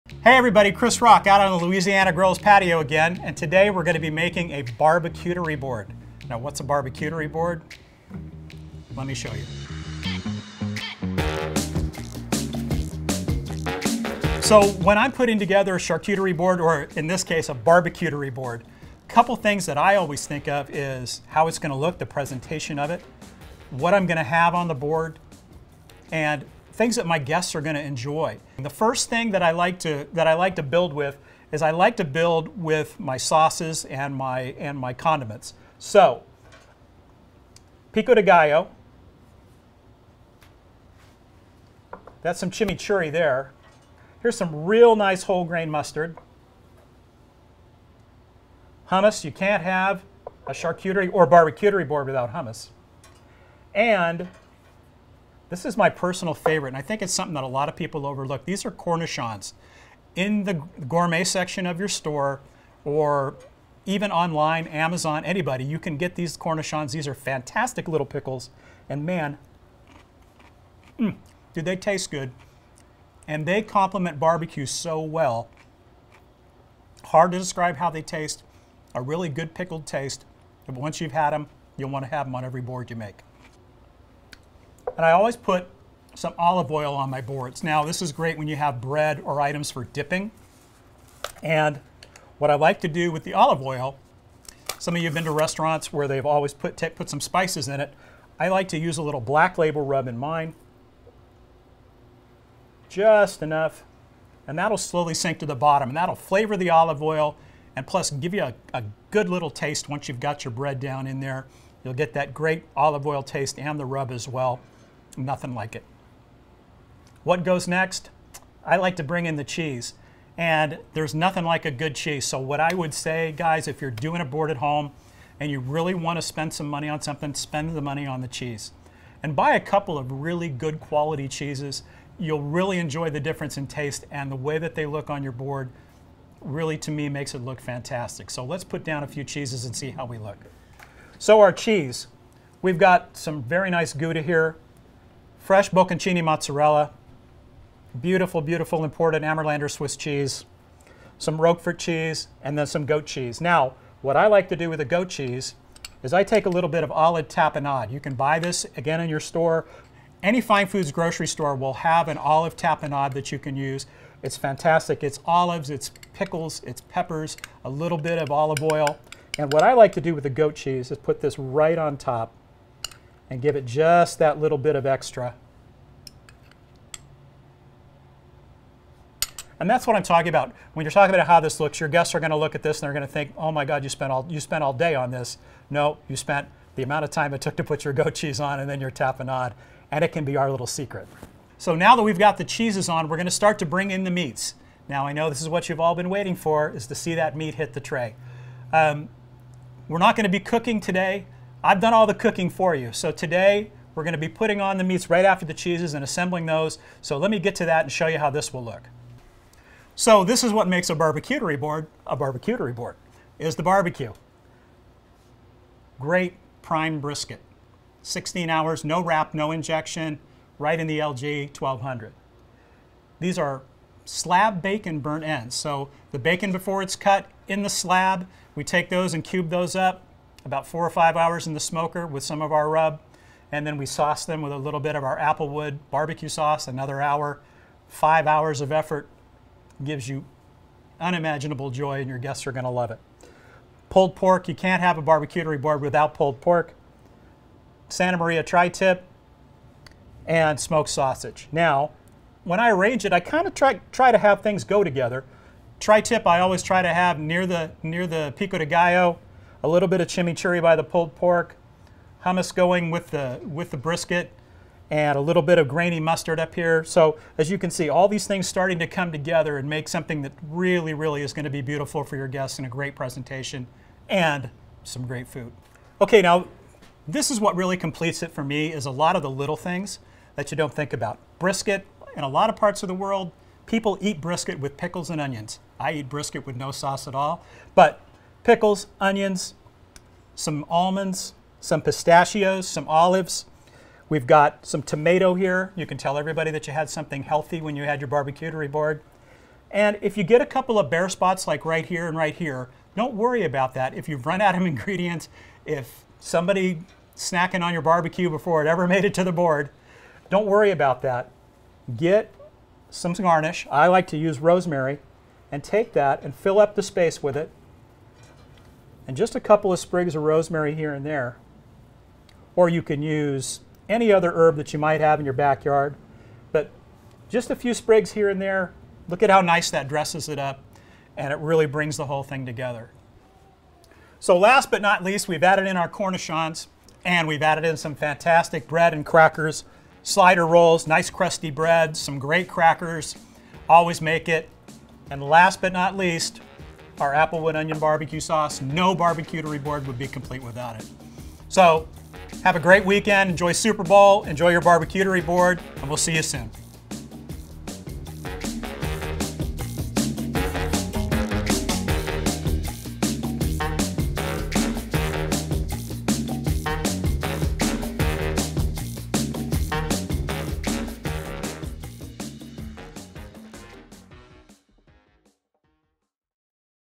Louisiana Grills® I The Patio I BBQterie Board Recipe
Enjoy this years Big Game with a BBQTeries Board full of your favorite smoked creations! 🏈

This flavorful display resembles a standard charcuterie board with a combination of your favorite sauces, condiments, and a variety of cheeses. The upgrade comes with the addition of smoked meats like pulled pork, brisket, and sausage!